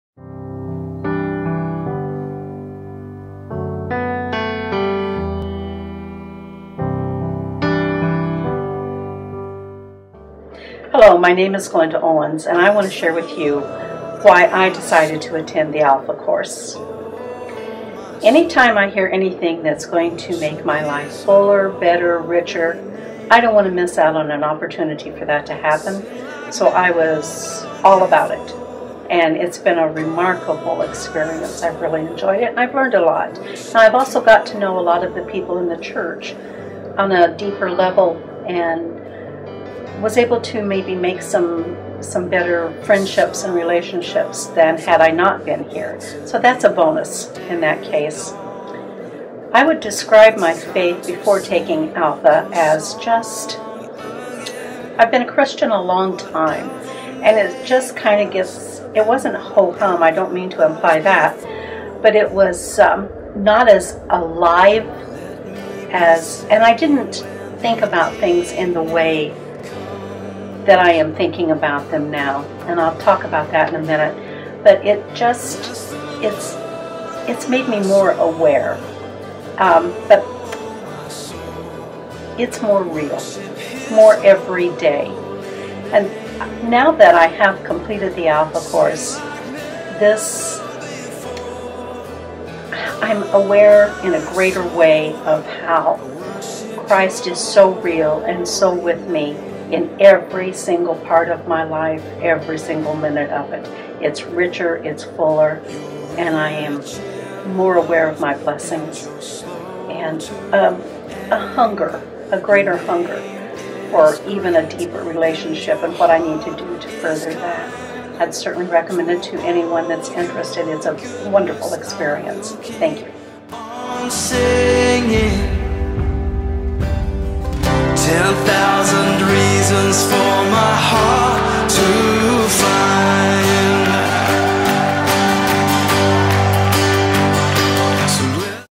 Alpha Project 2012 - Glenda Owens
Glenda Owens shares about her Alpha Course experience.